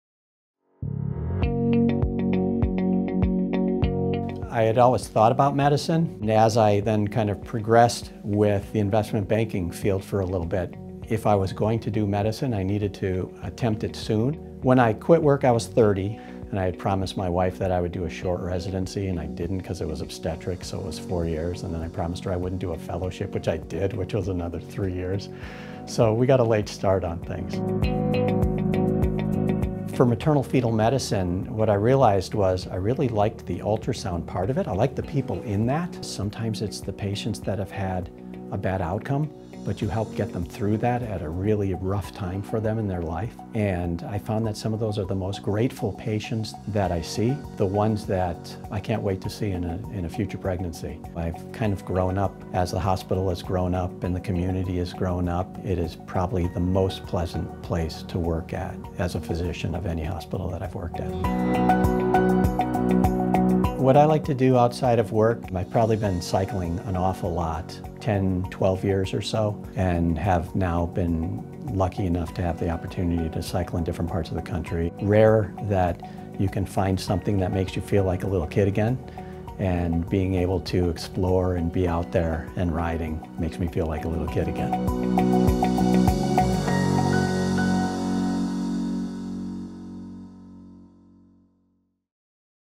Provider Spotlight: Michael DeRoche, M.D.
An investment banker out of college, he found a new calling with maternal-fetal medicine in his 30s and has been serving patients ever since.

Meet Dr. Michael DeRoche, a board-certified maternal-fetal medicine physician with Williamson Health Medical Group!